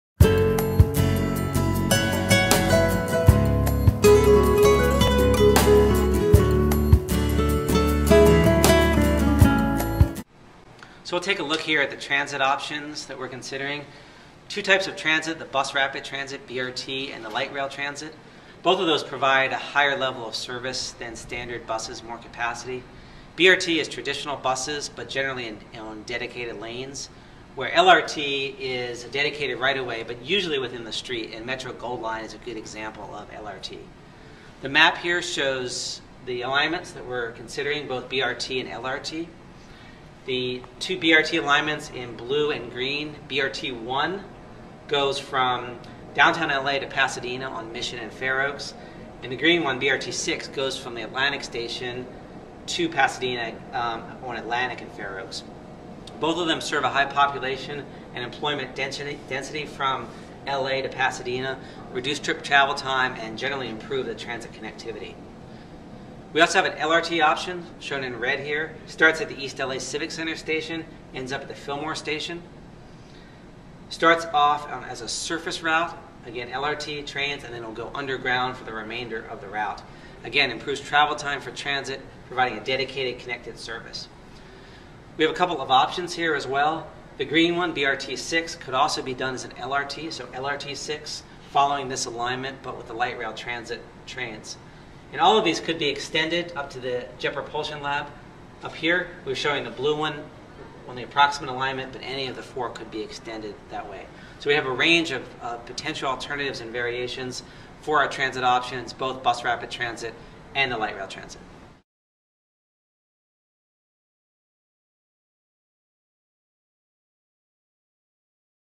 State Route 710 Study Alternative Concepts: Light Rail Transit/Bus Rapid Transit
Find out what's next for the State Route 710 Study and provide feedback. Metro is moving forward with the environmental review phase and the search for ways to address congestion in the State Route 710 Study area. This lack of connectivity of the freeway system in this area causes major traffic disruption not only across the communities around the State Route 710, but the entire region as well.

This video provides a brief overview of one of several Alternative Concept categories under consideration - Light Rail Transit/Bus Rapid Transit. Others under consideration are: Highway/Arterial, Freeway Alternative Concepts, Advanced Technologies and No Build.